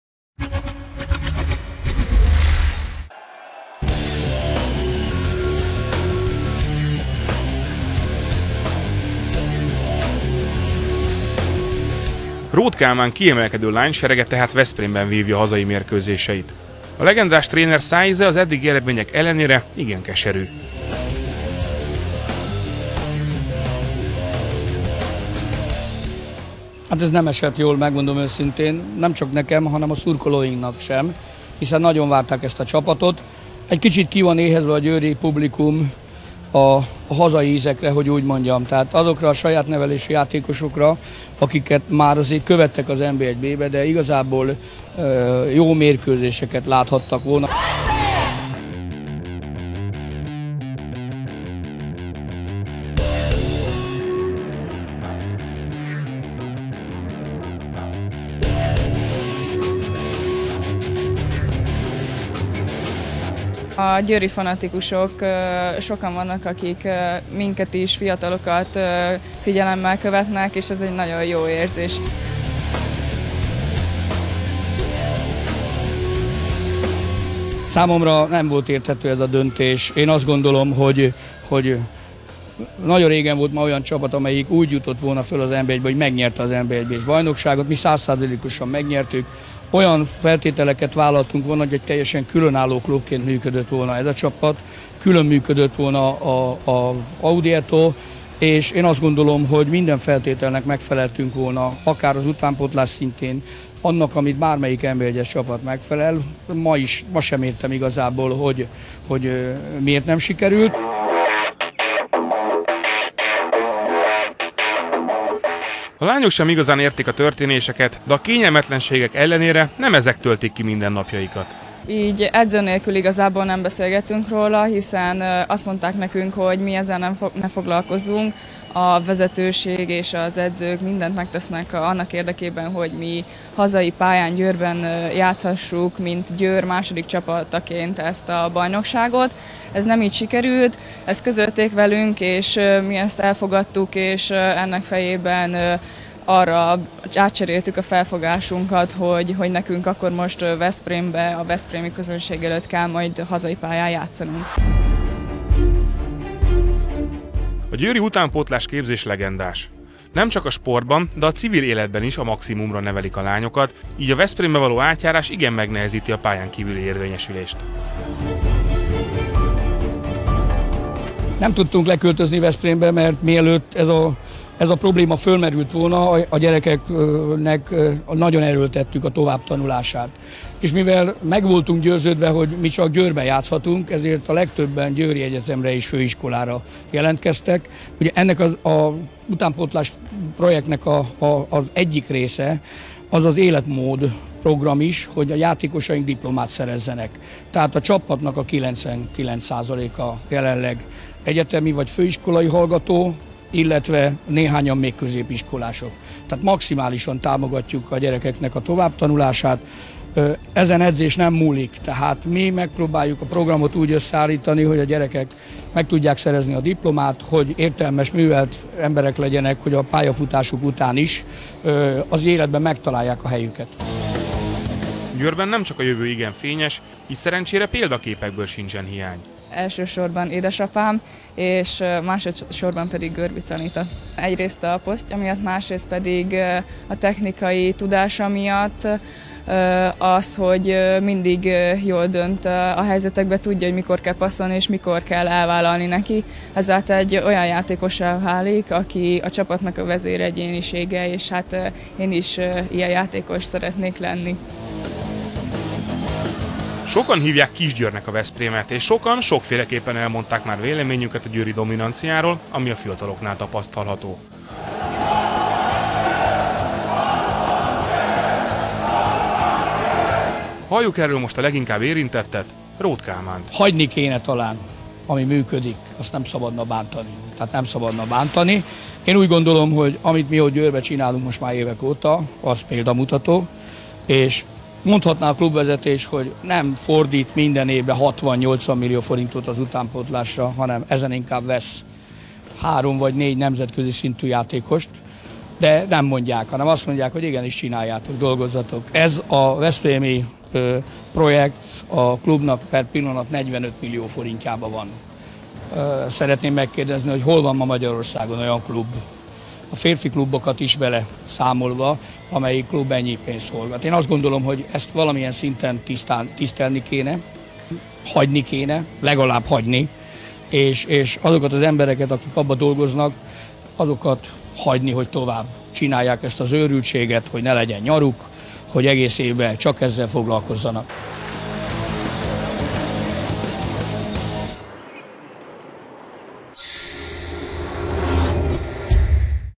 Otthon is idegenben?
Róth Kálmán Veszprémben is győri maradt, nem örül, hogy nem játszhatnak a Rába partján, de tovább tudja vinni a megkezdett munkát.